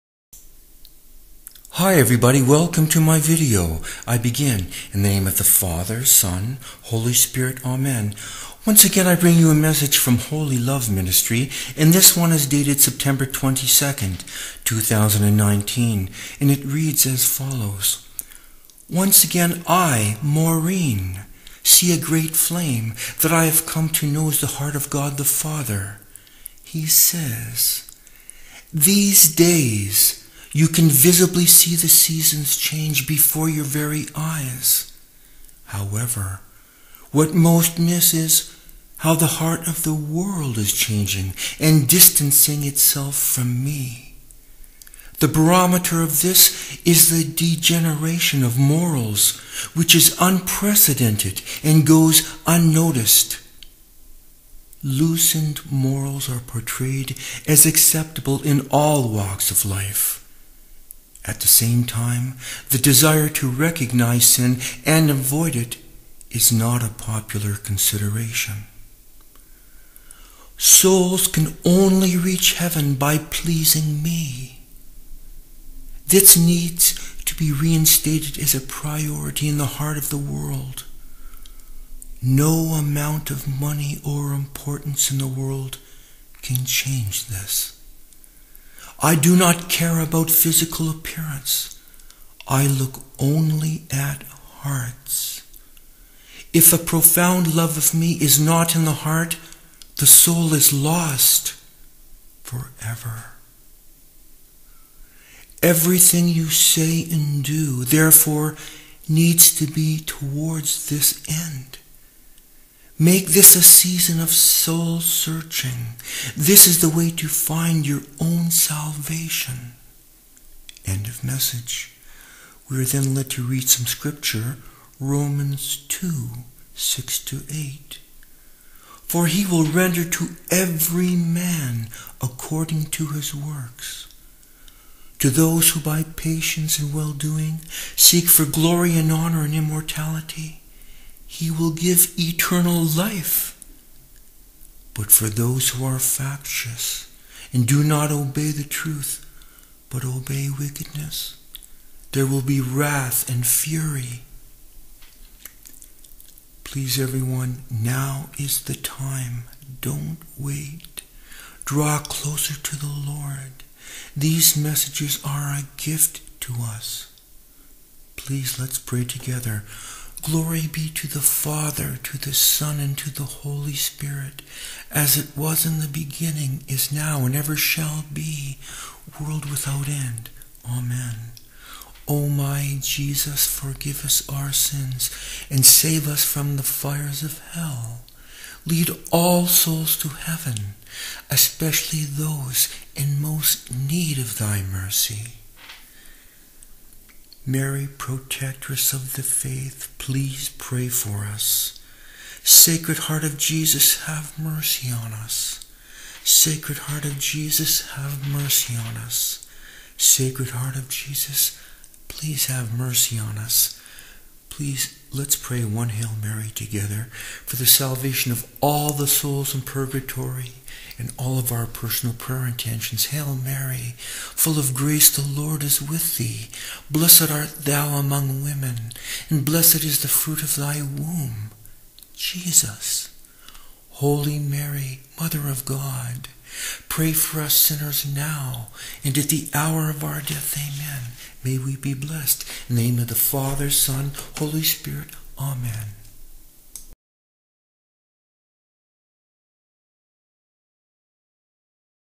Holy Love Messages September 22 and 21, 2019.
Once again, I (Maureen) see a Great Flame that I have come to know as the Heart of God the Father. He says:  "These days, you can visibly see the seasons change before your very eyes. However, what most miss is how the heart of the world is changing and distancing itself from Me. The barometer of this is the degeneration of morals which is unprecedented and goes unnoticed. Loosened morals are portrayed as acceptable in all walks of life. At the same time, the desire to recognize sin and avoid it is not a popular consideration."

"Souls can only reach Heaven by pleasing Me. This needs to be reinstated as a priority in the heart of the world. No amount of money or importance in the world can change this. I do not care about physical appearance. I look only at hearts. If a profound love of Me is not in the heart, the soul is lost forever. Everything you say and do therefore, needs to be towards this end. Make this a season of soul-searching. This is the way to find your own salvation."

Read Romans 2:6-8 +
For he will render to every man according to his works: to those who by patience in well-doing seek for glory and honor and immortality, He will give eternal life; but for those who are factious and do not obey the truth, but obey wickedness, there will be wrath and fury.

Once again, I (Maureen) see a Great Flame that I have come to know as the Heart of God the Father. He says:  "Collusions are forming now - coming together towards an evil end, which will come to light in the near future. Much evil has been clothed in goodness. My children who have naively trusted in titles - will awaken too late. True wisdom is confounded by the desire to please self and others."
"My children need to be assertive in the Truth. The time is coming when My Remnant Faithful will need to make themselves known. Even the littlest souls who remain hidden in prayer will need to, in holy boldness, stand for the Truth in the face of error."
"Complacency is the enemy of the true Faith. The genuine prayer warriors need to convict errors without the fear of reprisals. Entrust the fear of public opinion to My charge. Know that My Paternal Love embraces you. Everywhere you turn is My Will. I can change circumstances which will expose what lies hidden in hearts. I can change the effects of certain events. Therefore, never fear - but trust."
Read Psalm 4:1-3 +
Answer me when I call, O God of my right! You have given me room when I was in distress.
Be gracious to me, and hear my prayer.  O sons of men, how long will you be dull of heart?
How long will you love vain words, and seek after lies? But know that the Lord has set apart the godly for himself;
the Lord hears when I call to him.